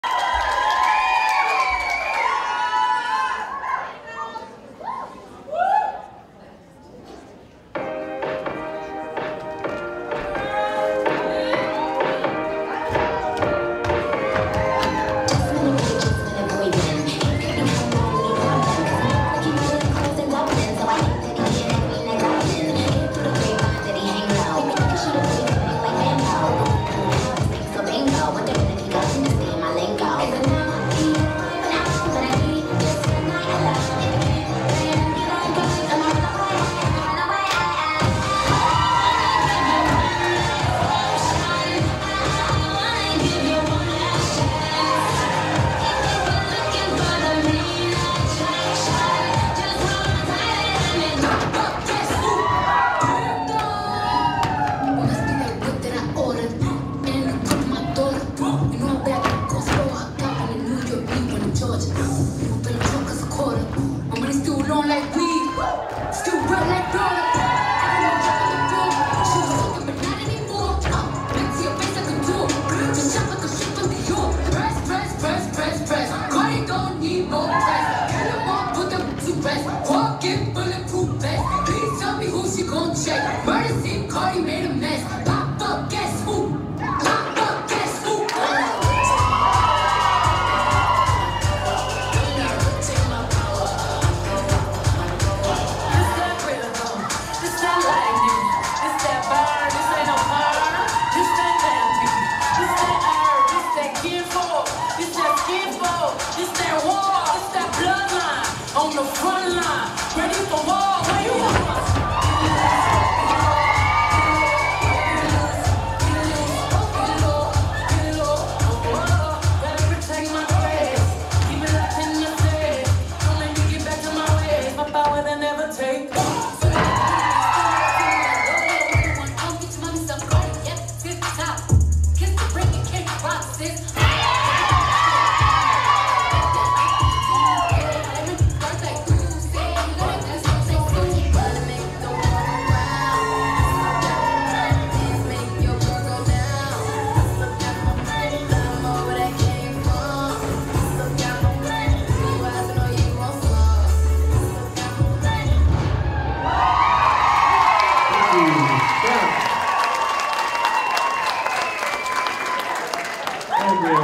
Femme - Teen Hip Hop Group 2024
2nd Overall Teen Small Group
Hip Hop Group Age 14
Choreo - Alyssa Aceti
Pure Rhythm Dance Co